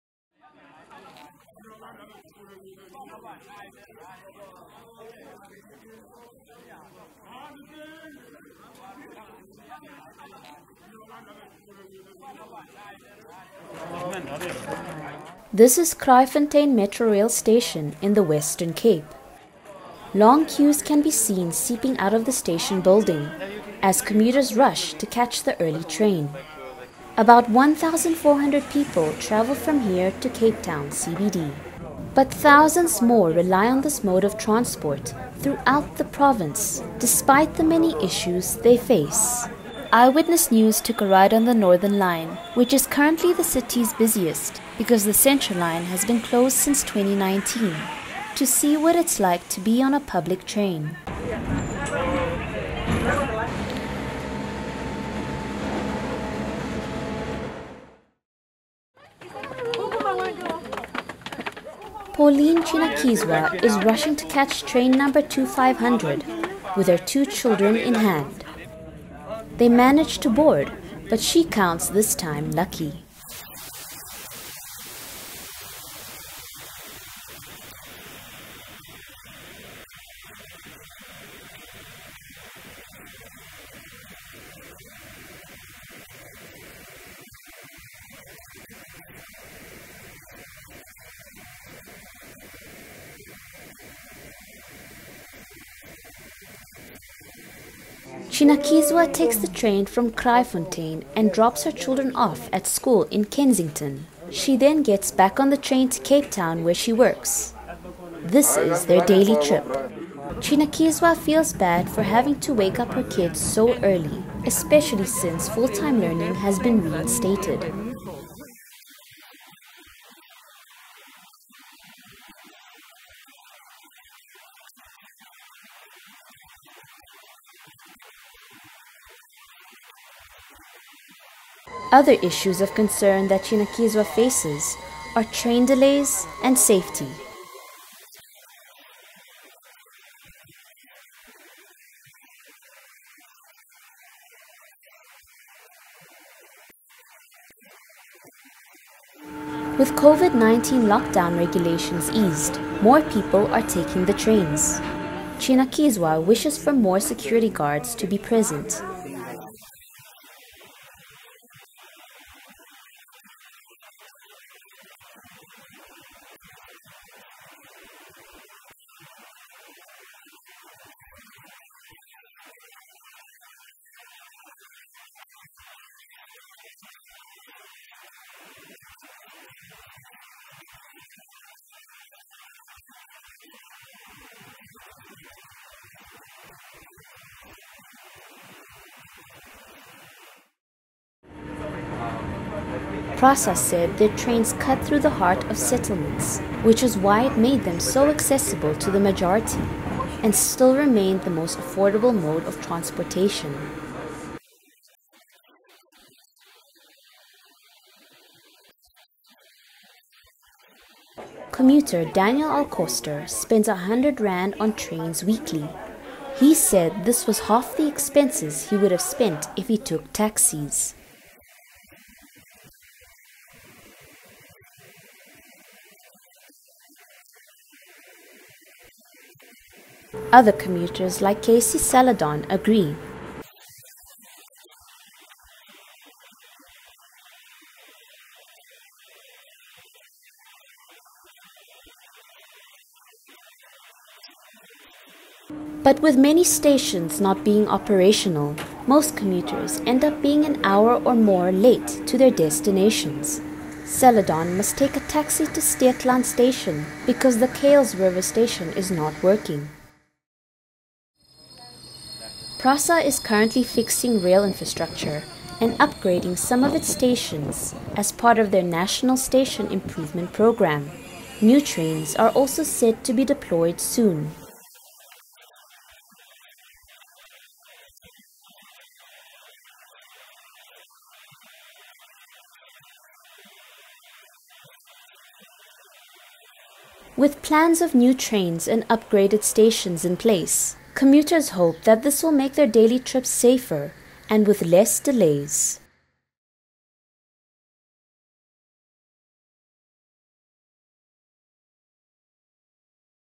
What it’s like riding the Cape Town Metrorail train
With the return to office, full-time learning reinstalled and petrol price increases - the reliance on public trains has increased. Eyewitness News took a trip on one of the busiest lines in the Western Cape to uncover the experiences of train commuters.